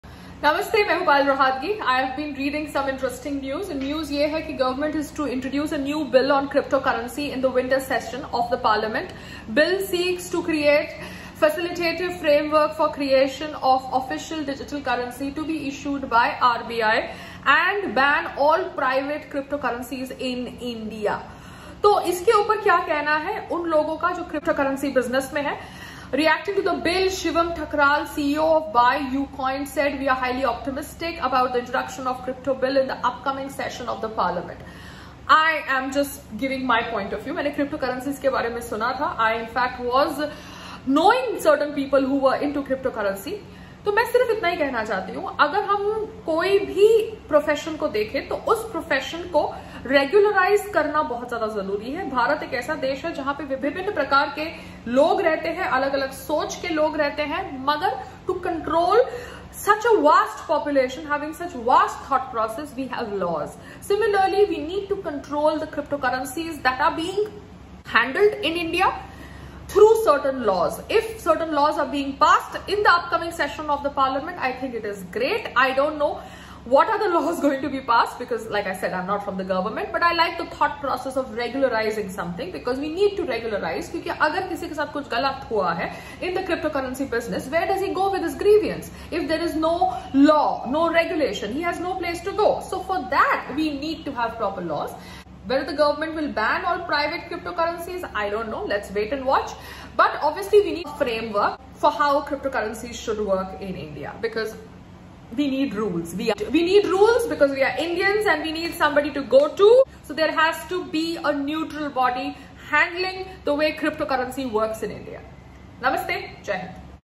हम भारतीय हैं हमें rules चाहिए 🤪 cryptocurrency - Payal Rohatgi
हम भारतीयों है और हमें आवश्यकता है rules की क्योंकि हम बहुत मुश्किल से rules का पालन करते हैं और अगर rules ही होंगे तो इतने बड़े देश में हलचल मच जाएगी। इस देश के अंदर cryptocurrency का धंधा शुरू करने का बहुत प्रयास किया जा रहा है और इस प्रयास में ग़लत ना हो मासूम लोगों का पैसा ना डूबे उसके लिए Sarkar अगर कोई नियम क़ानून लाना चाहती हैं तो cryptocurrency को regulate करने के लिए तो ये सही है।

मैं Sarkar का समर्थन इस मामले में करती हूँ।